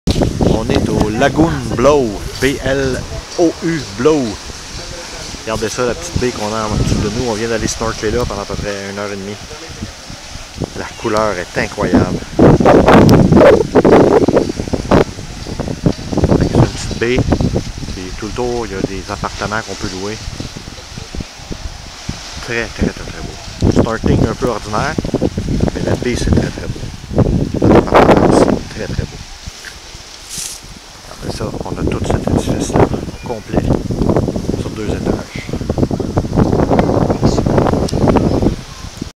Curaçao en février 2016
visitez - Notre appartement (maison) à Curaçao au Lagun Blou et la vue sur Playa Lagun